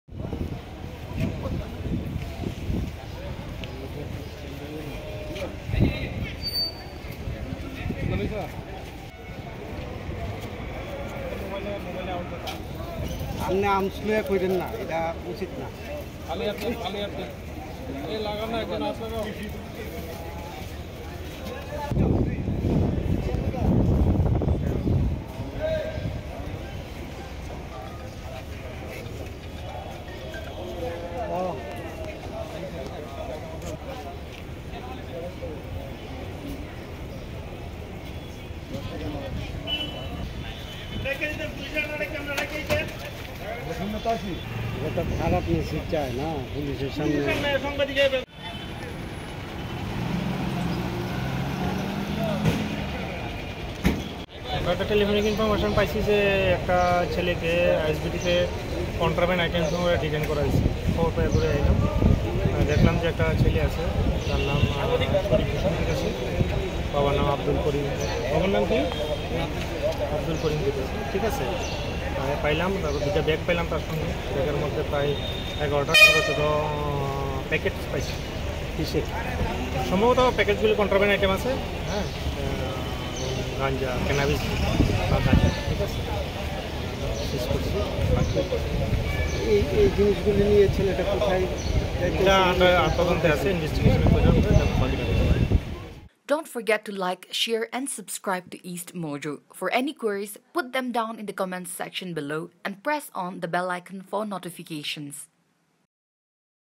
Tripura: Public helps nab drug mule in Agartala
The alertness of the public at the Inter-State Bus Terminal in the Chandrapur area of Tripura’s Agartala helped nab a drug mule on Sunday. It was after the movements of a youth who had bought a ticket to travel to Guwahati, that raised suspicion among the motor vehicle workers present at the bus terminal. The youth was then intercepted and after searching, several packets of marijuana were found in his luggage. Soon police were informed and the youth identified as Sharif Hussain was handed over to the cops. Police later informed that Hussain carrying around 10 kilograms of marijuana in 11 packets. A case has been registered against Hussain and further investigation is underway.